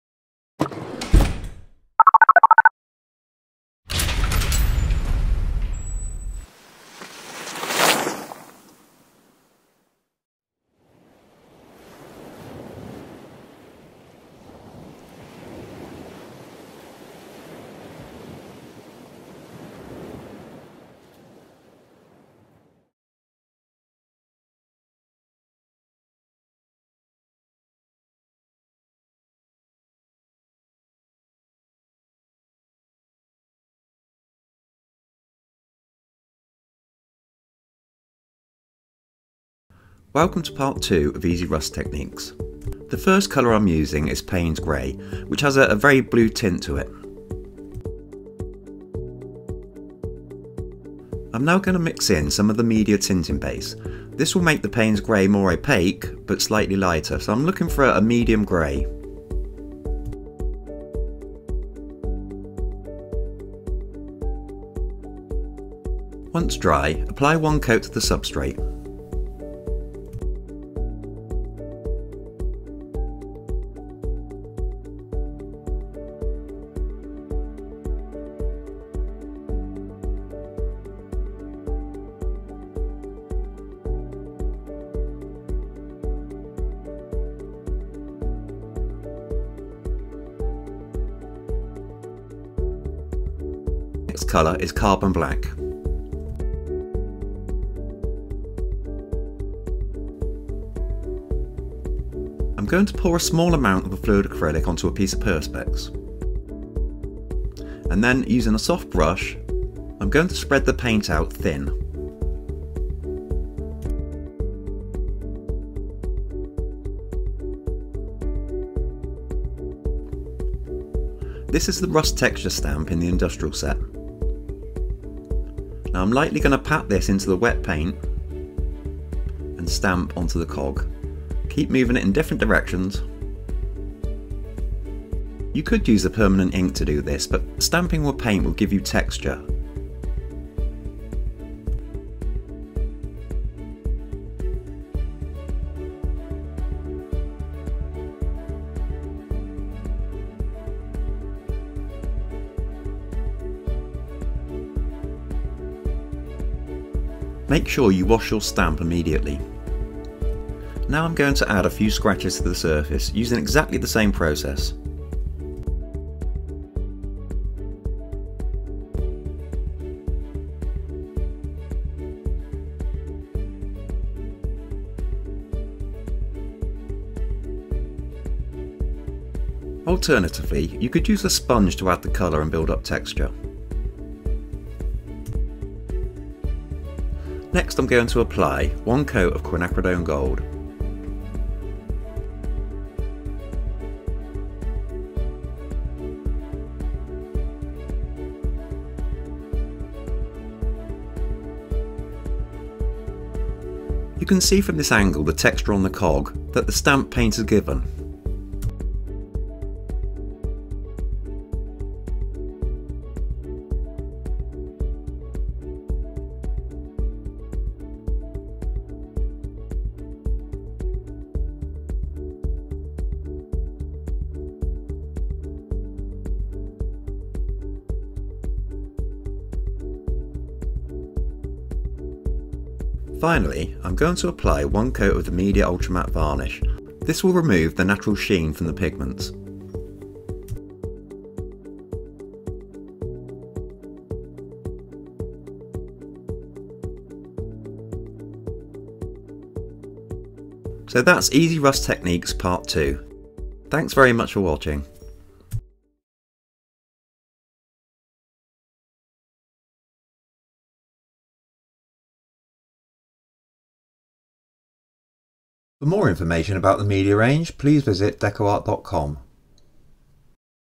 Easy Rust Painting Part 2
Part 2 of a series of video tutorials with Andy Skinner, showing easy techniques for painting faux rust effects using Decoart Media Fluid Acrylics and my Stampendous industrial texture stamp set.